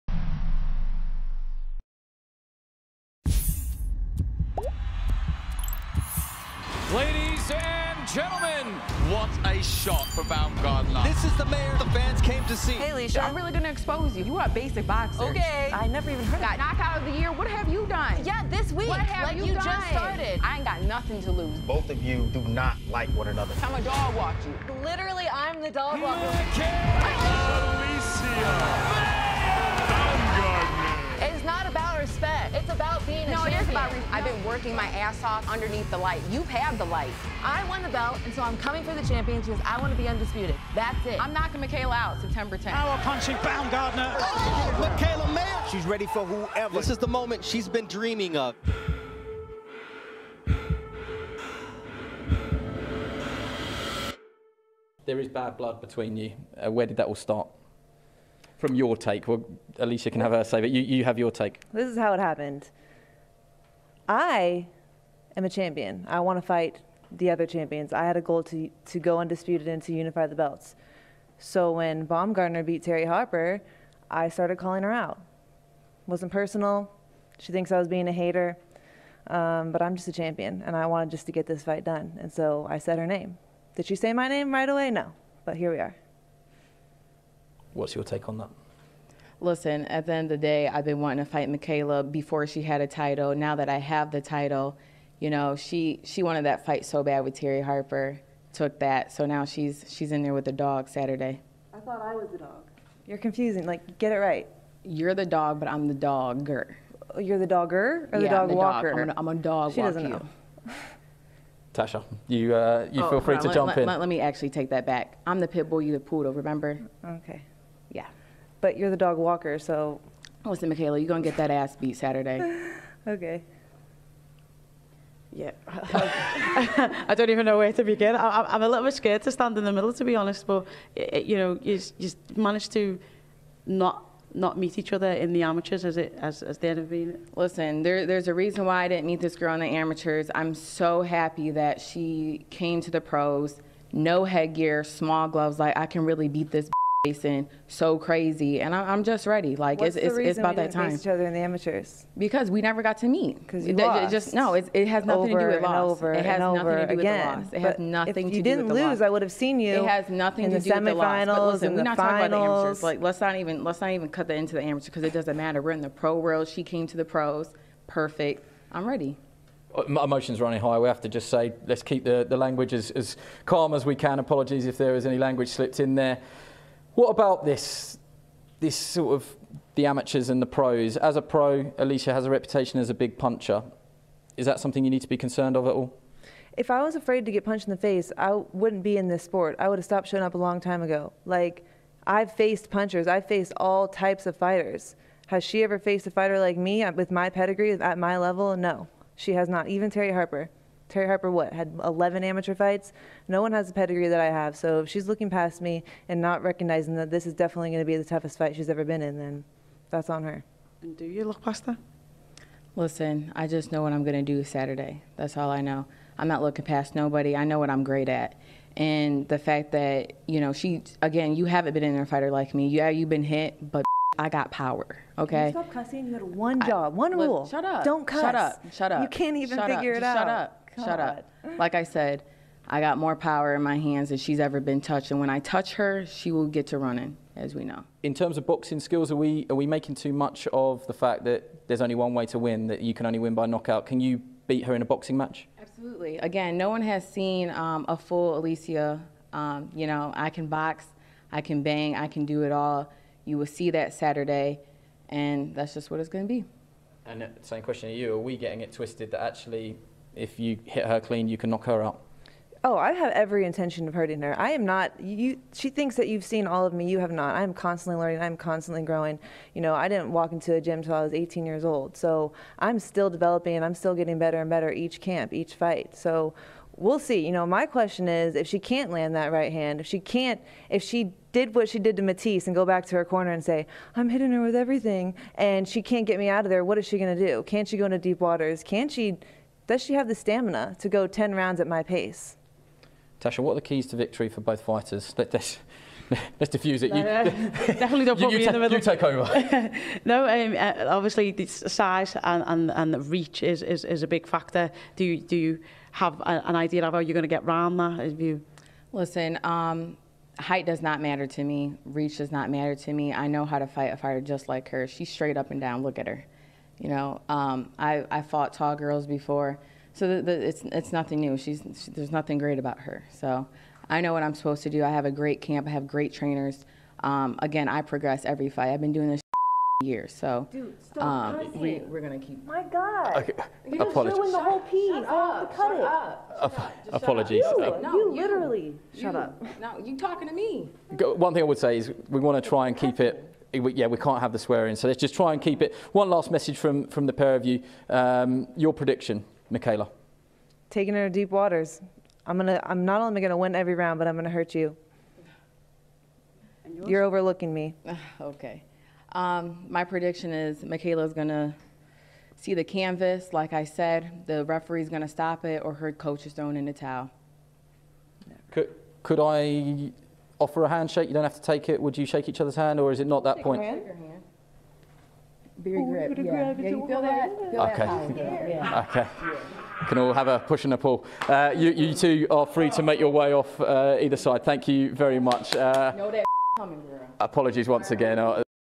(Heated Exchange) Mayer vs Baumgardner
On the undercard of Shields vs. Marshall , the rivalry between Mayer and Baumgarder will be settled but before the fight The Trash Talk.

Credit: Sky Sports, Dazn

APPRECIATE THE LOVE FAM

COME THRU AND CHILL, SEE YOU AT THE STREAMS